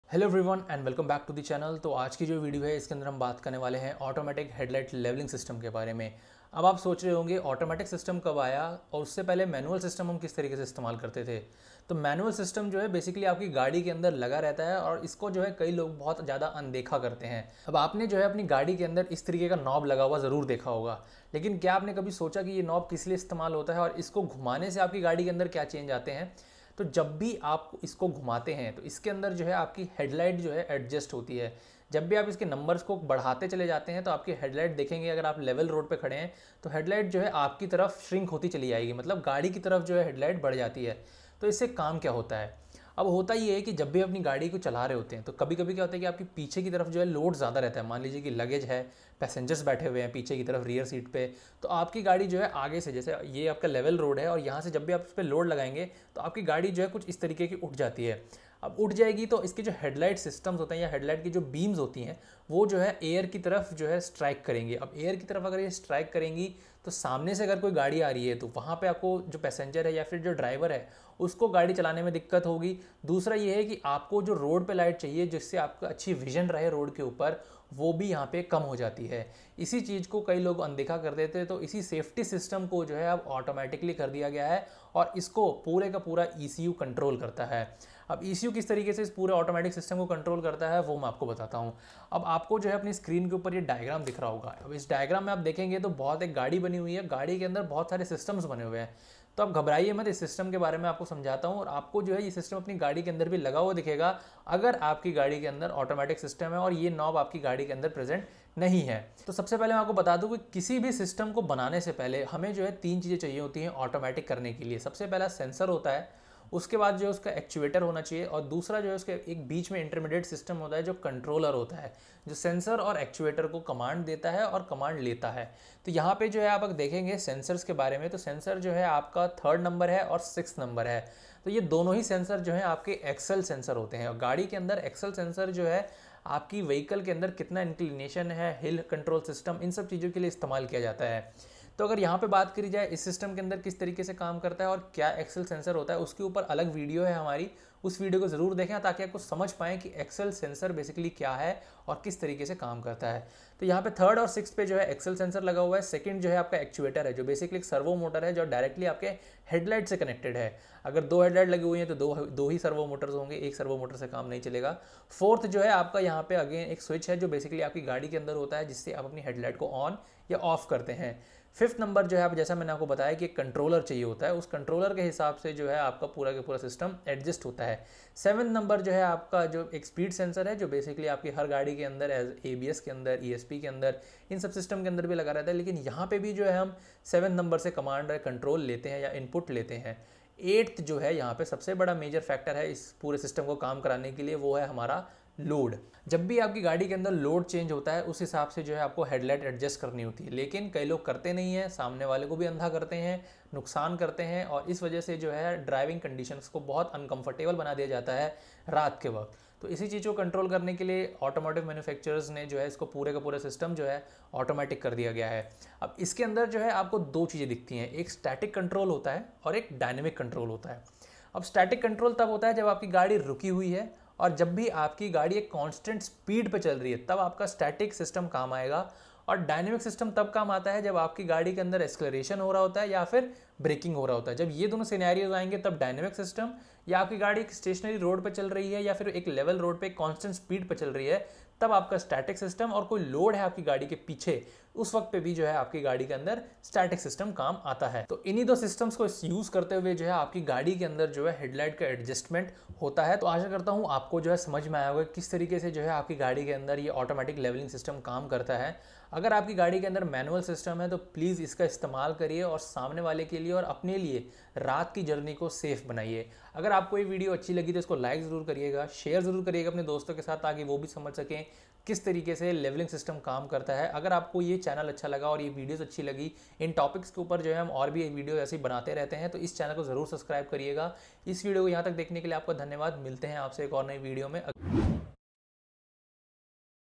How Headlight Leveling Technology work | Automatic Headlight system
Automatic Headlight system help in adjusting the headlight bean rays according to the change Lin level of your vehicle. For proper safety while driving one road we need to change light ray striking on road. So that proper vision can be maintain and driver get proper vision while driving in night. It a request from our Team is, if you won a vehicle with manual system please lean how you can adjust these beam according to the change in load. You can look into your car manual for proper weight system. In common language its more about headlight angle adjustment.

Team TNT
Shubham ojha